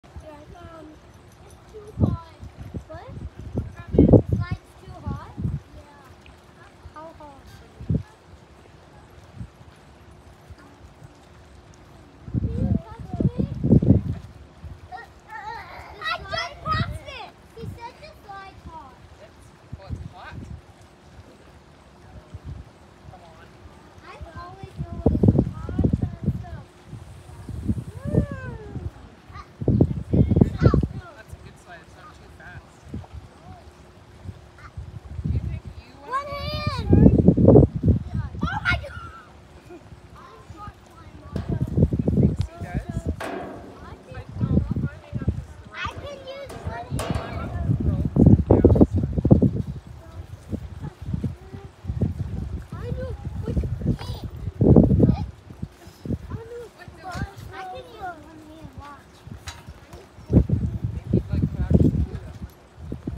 A good day to play.
Jaxon & Bryce enjoying the harbour with Meme.